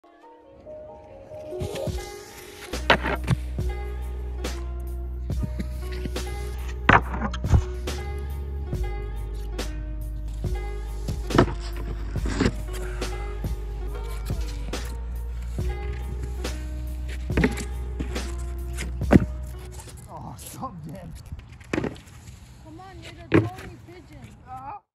Tony Pigeon Snowskating
Finally able to dust the ol' Snowskate off and get back at it.